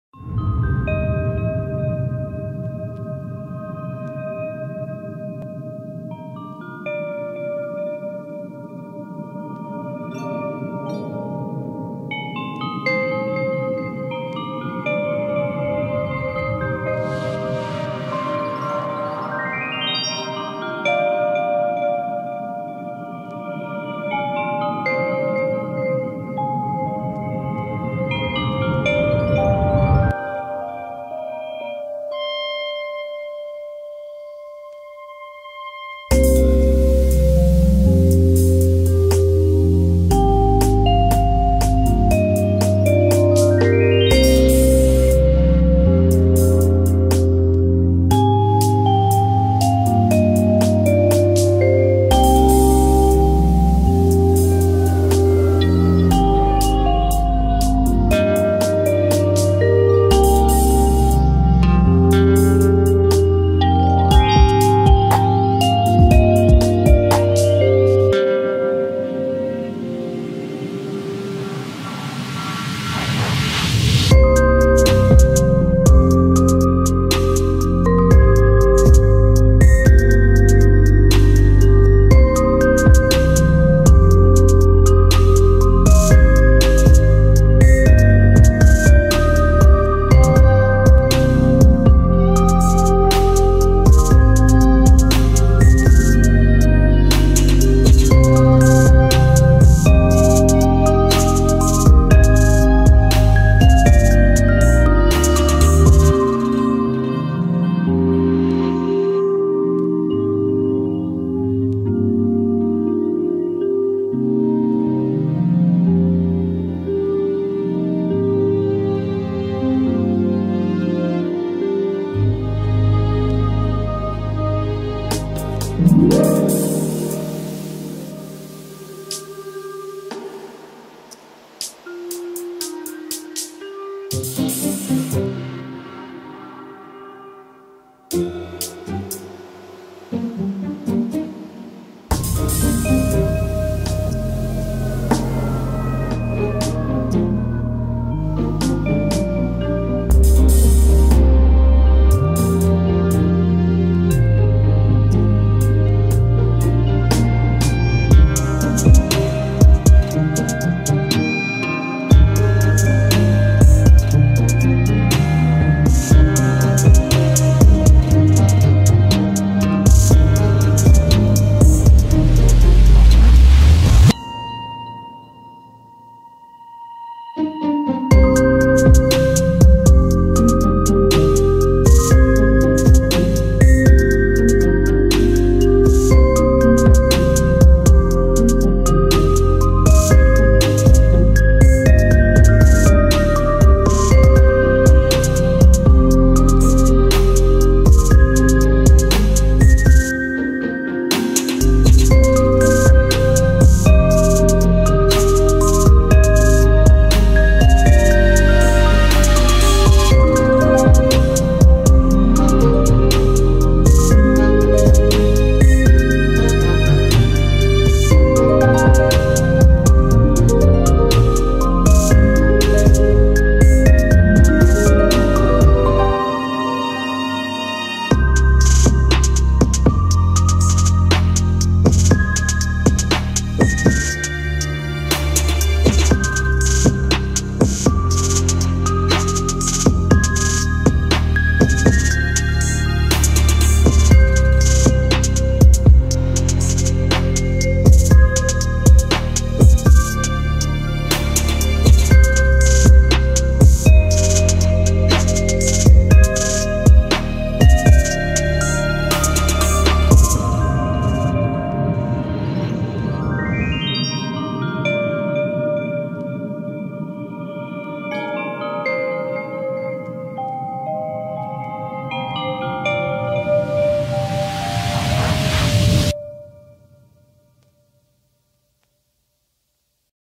Медитация Релакс Пузыри Наполнят Ваше Тело
Медитация Релакс Пузыри Наполнят Ваше Тело и сердце Помогут Погрузиться глубоко в себя